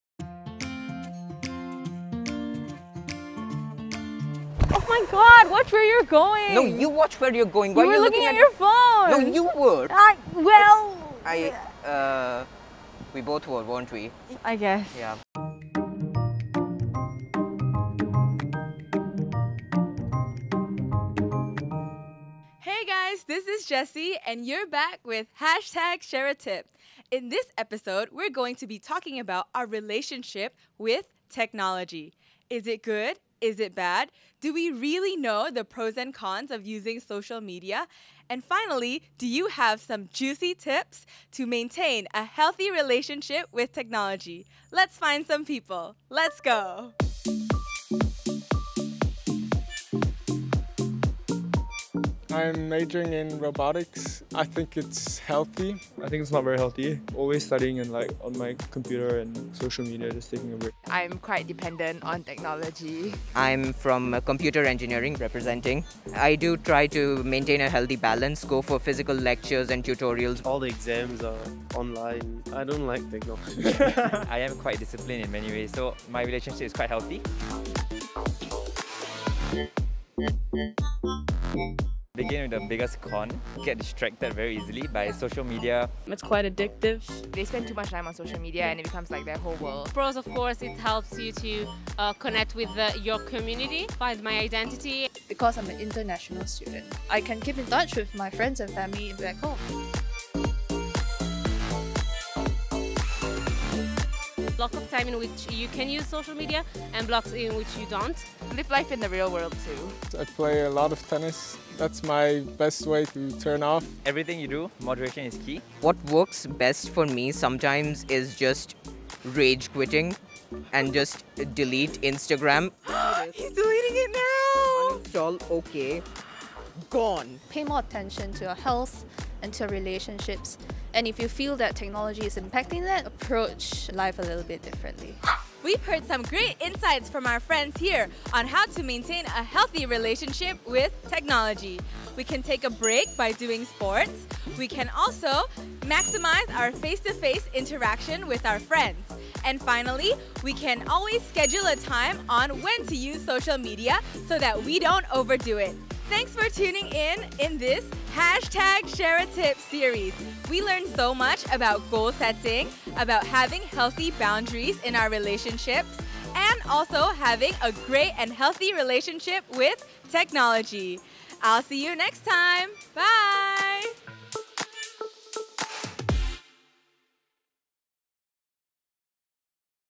ShareATip - How healthy is your relationship with tech?
Being a busy student during the pandemic can be extra stressful. The Office of Student Affairs launches the ShareATip series to brighten your student life with these useful wellbeing tips. Hear what your peers have to say!